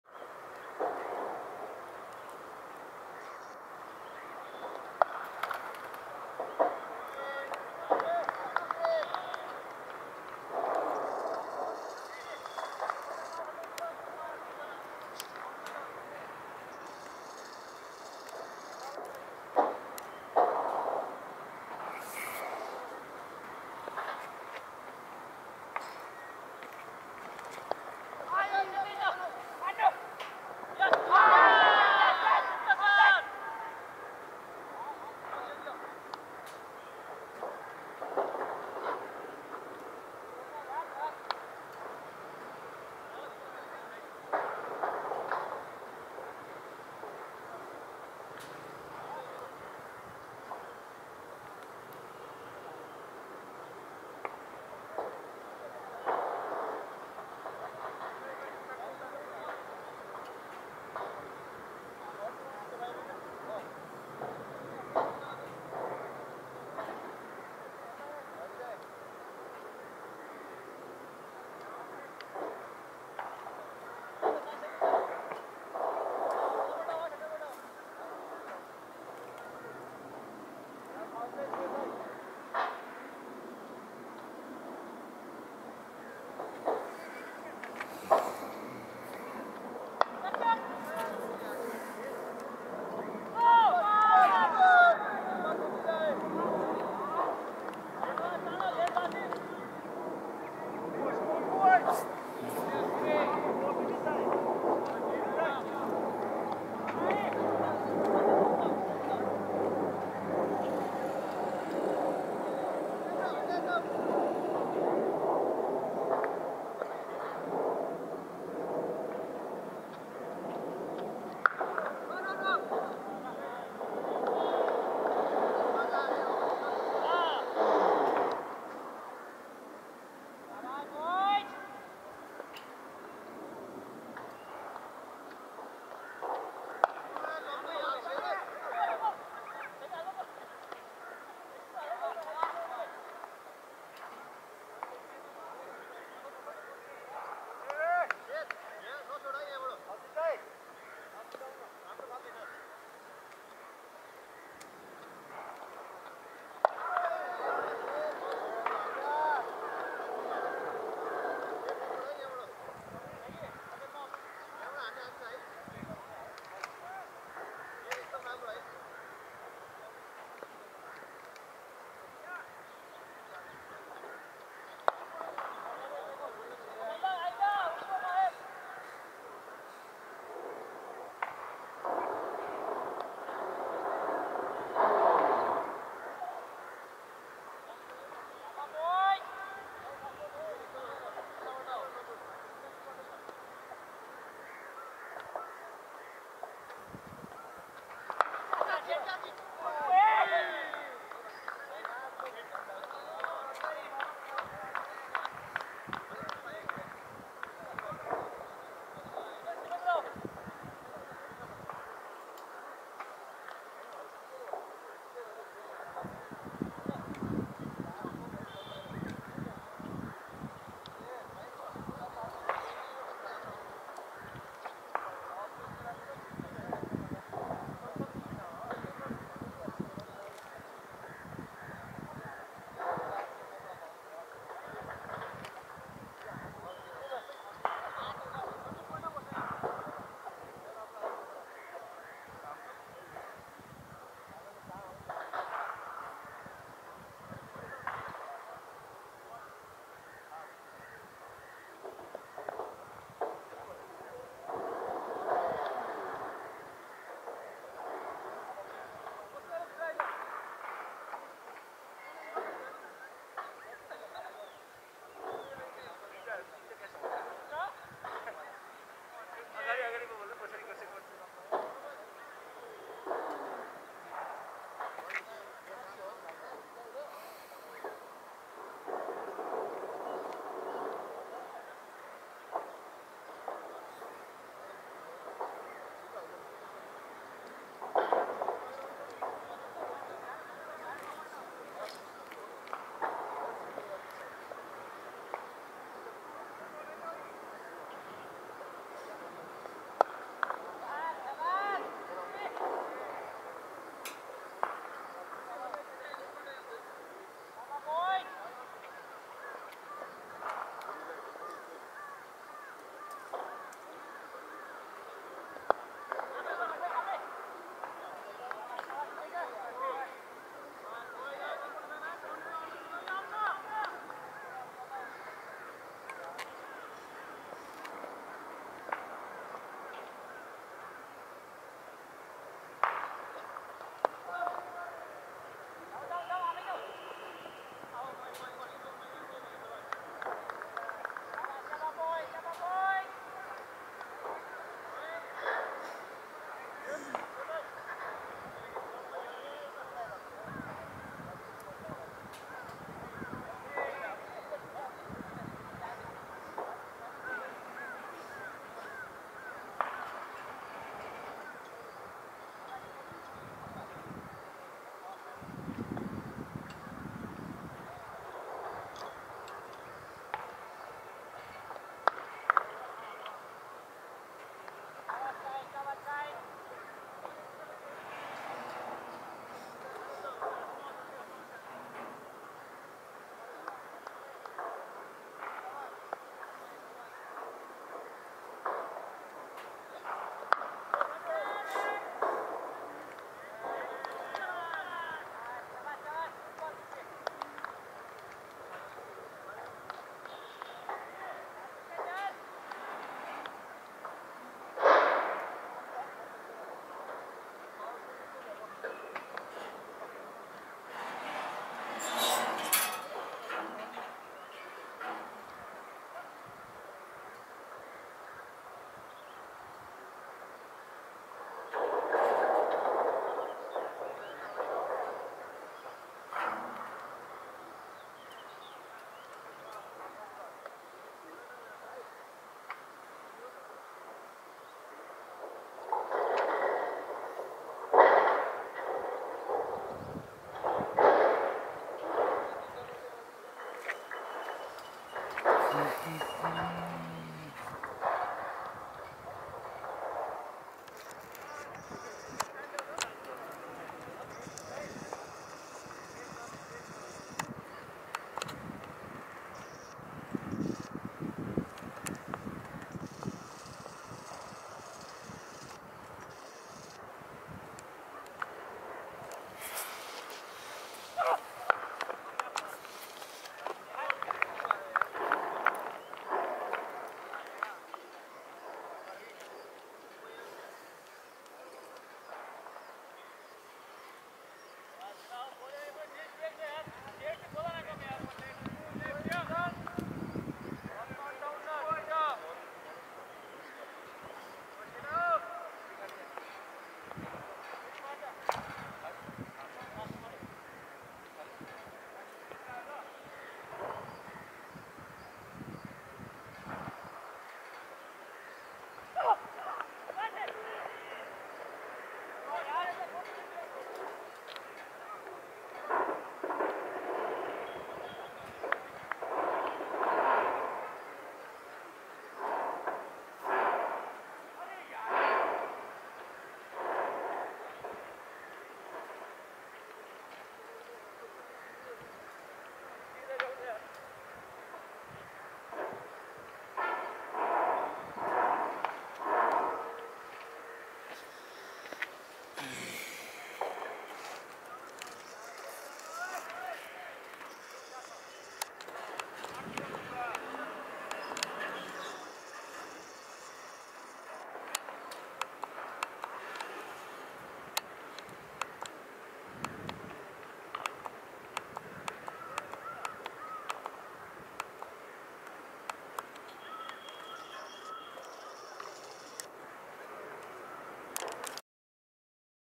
Nepal Cricket Team Practice match for T20 with Ireland | Sandeep Jora Team Vs Rit Gautam Team
Nepal Cricket Team Training for T20 World Cup | Kamal Singh Airee bowling to Sompal Kami
Location Mulpani
Copyright@AdamVlog